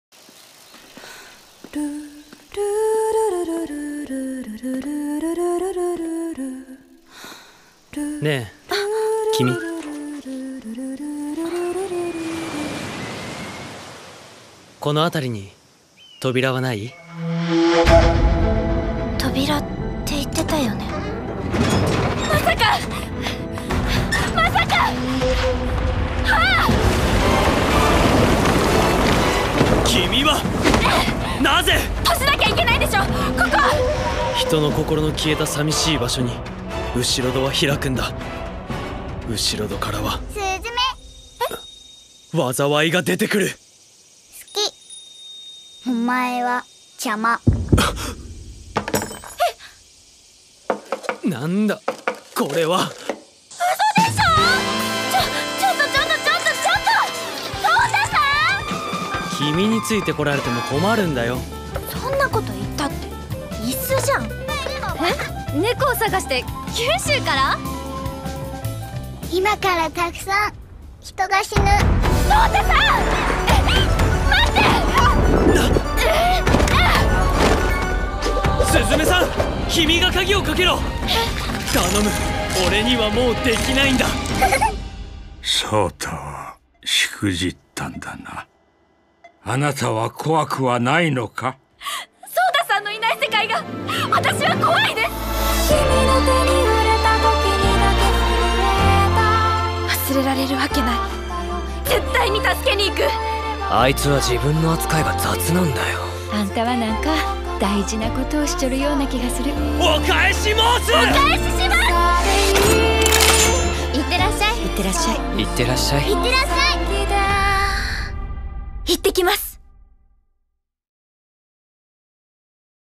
🌧 The final running time of the film "Suzume no Tojimari"
🌧 The final running time of the film "Suzume no Tojimari" (Suzume that closes the doors) from the cloud drawing champion with trains Makoto Shinkai will be 2 hours and 1 minute. Will launch the picture on November 11th.